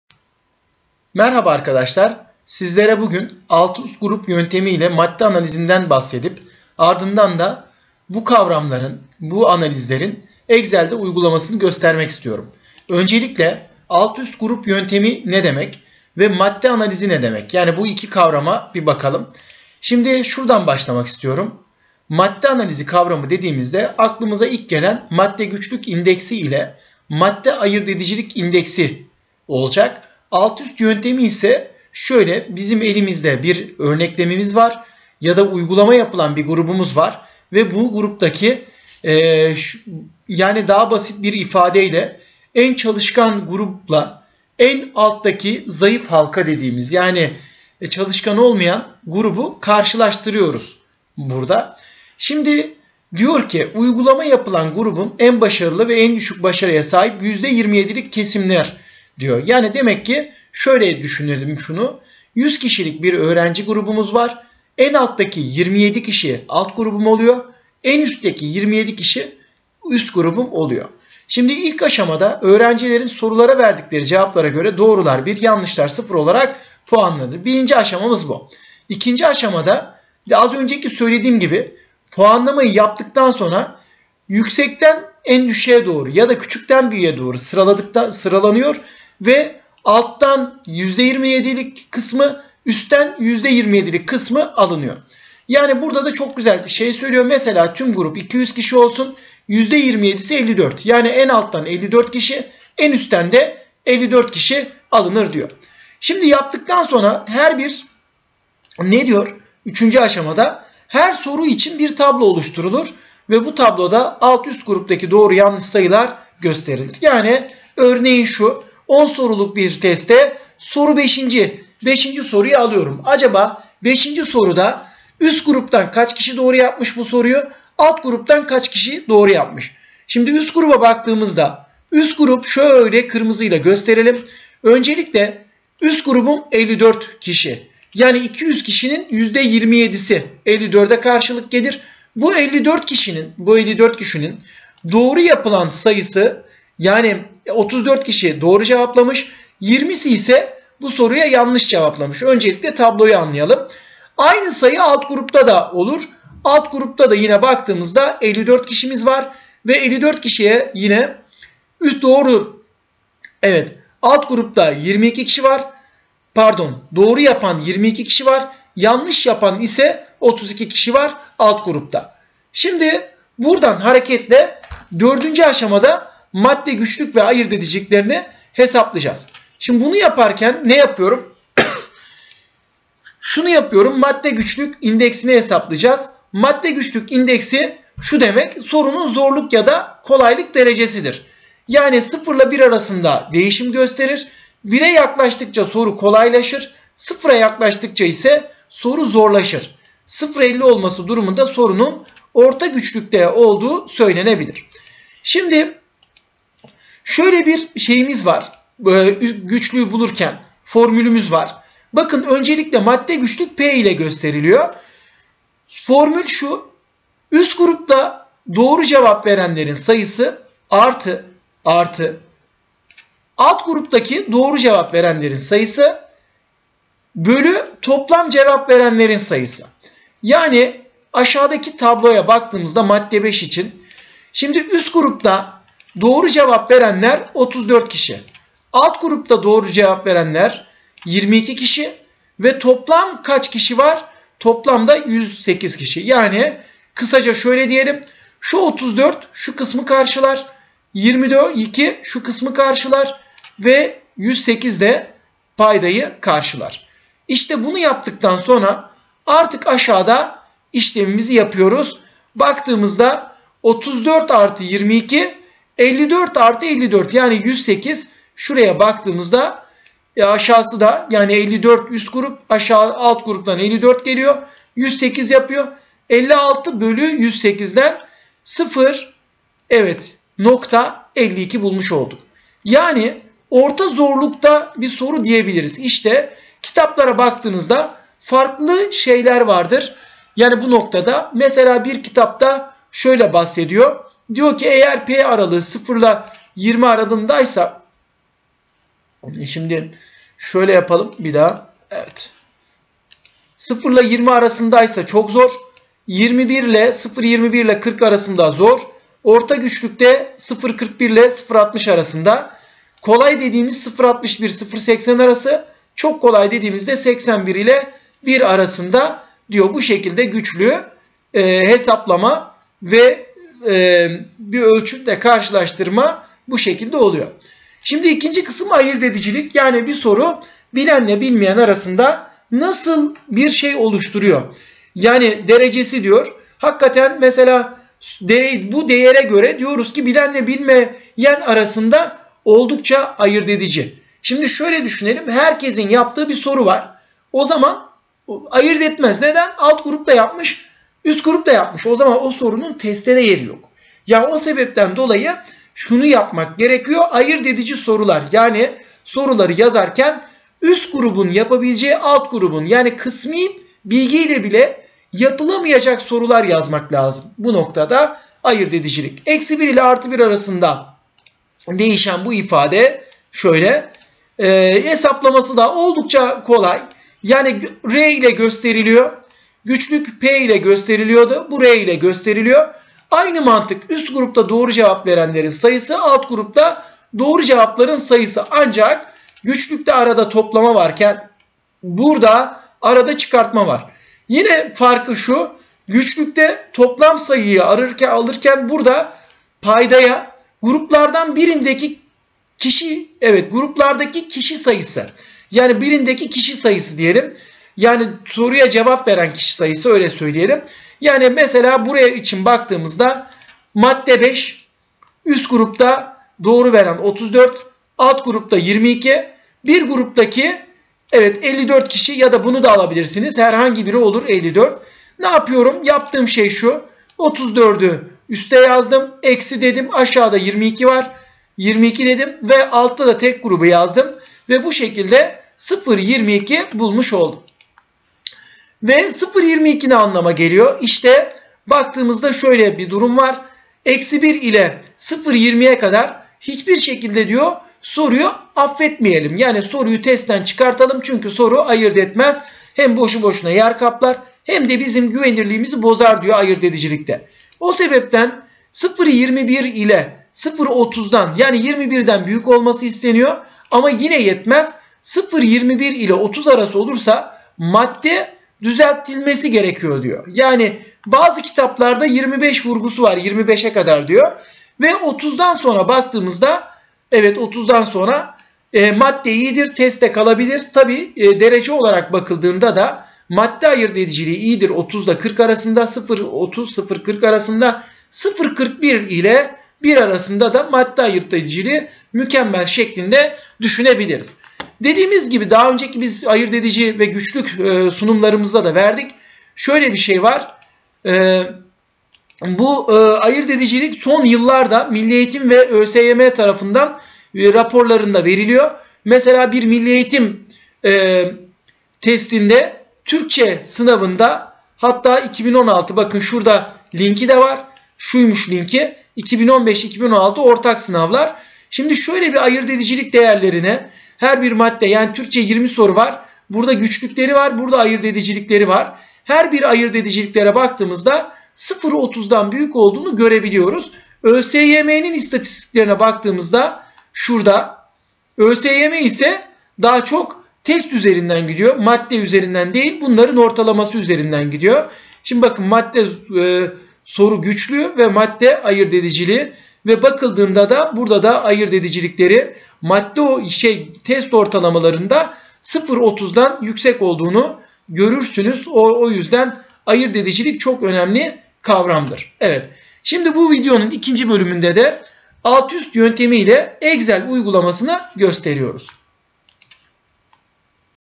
Excel ve Alt - Üst Grup Yöntemi ile Madde Analizi 1
İki video olarak hazırlanan bu bölümde alt-üst grup yöntemi ile madde güçlük/madde ayırt edicilik indekslerinin nasıl hesaplandığı excel üzerinde gösterilmiştir.